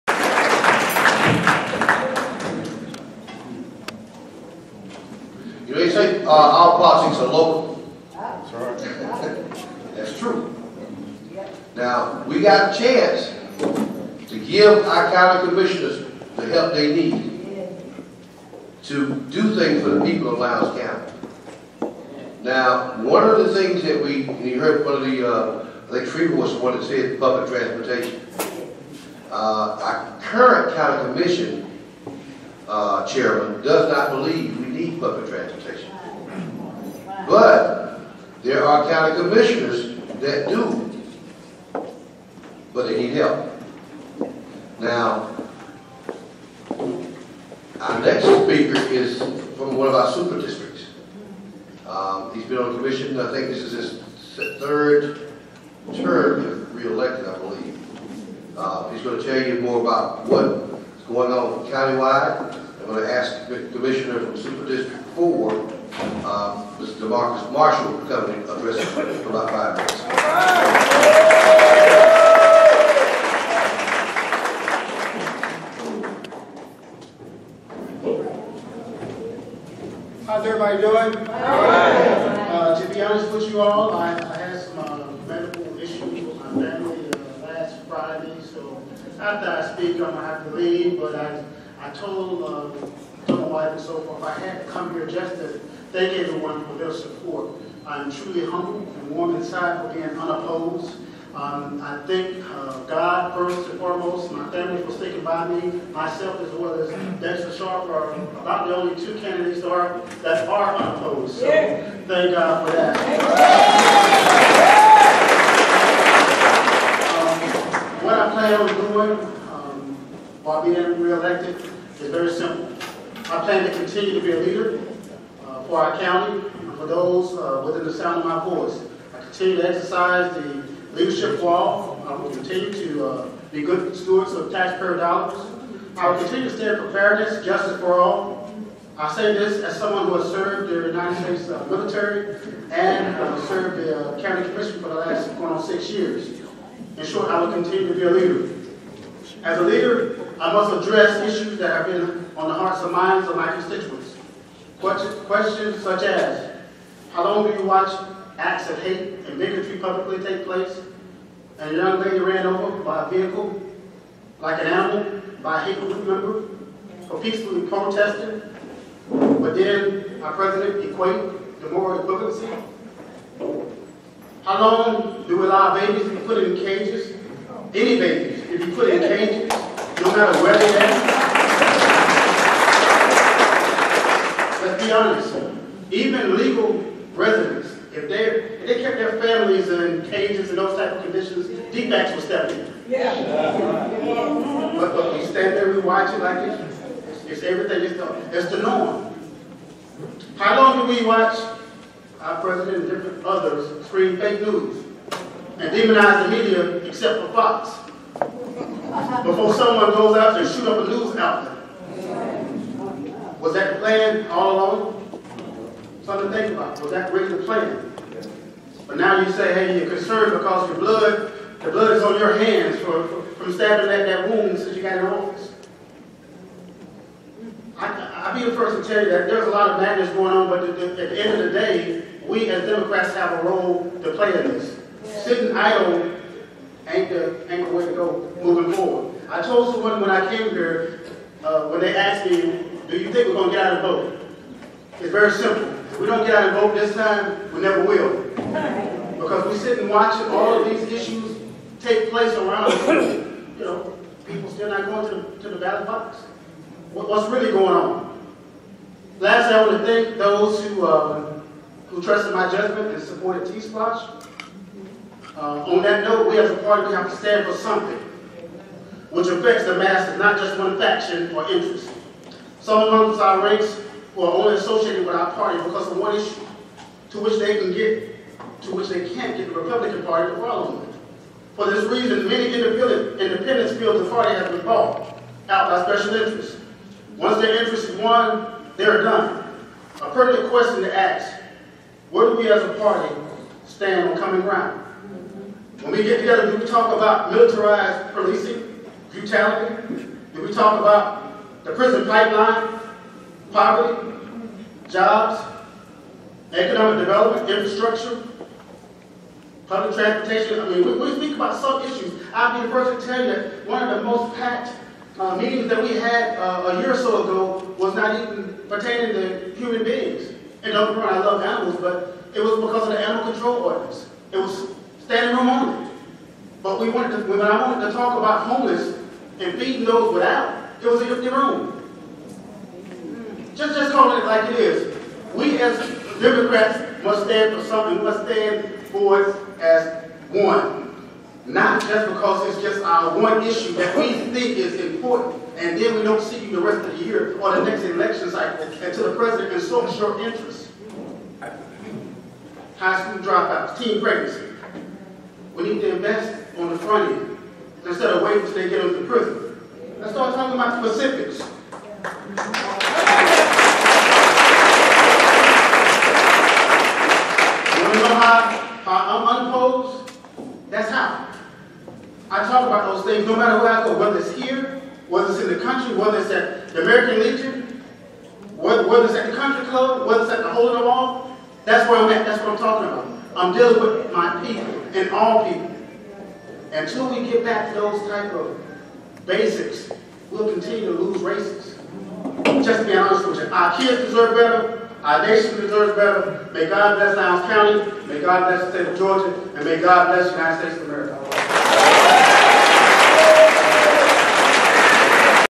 Commissioner Demarcus Marshall (Lowndes District 4) @ LCDP BBQ - 2018-07-02
LCDP Annual BBQ - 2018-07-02 - Ride the Blue Wave
Mathis Auditorium - Valdosta GA